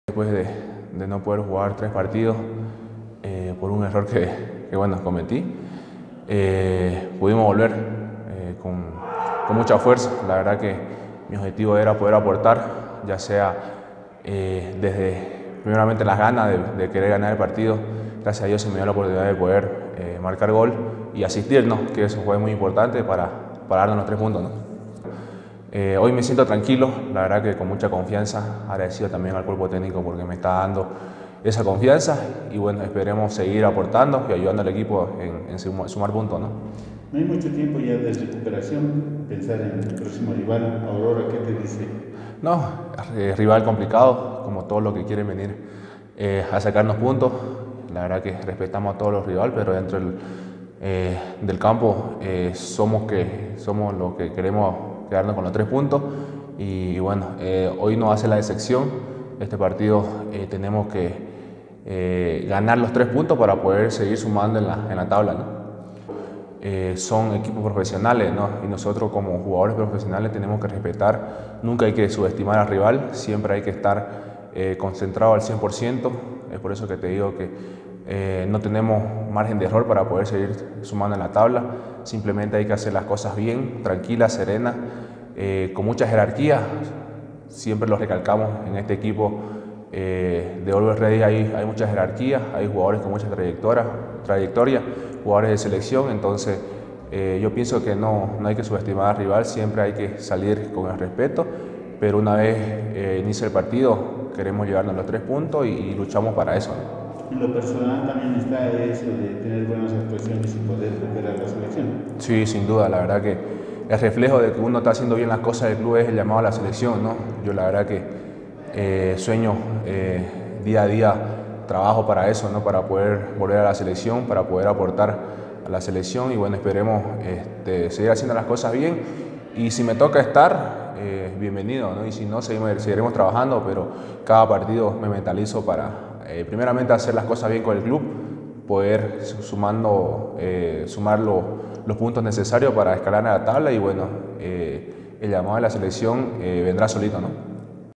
Always Ready vs Aurora hoy en Villa Ingenio I Carmelo Algarañaz
Carmelo Algarañaz brinda sus impresiones sobre el partido que los alteños jugarán hoy sábado desde las 15h00 frente a Aurora en el Estadio Municipal de El Alto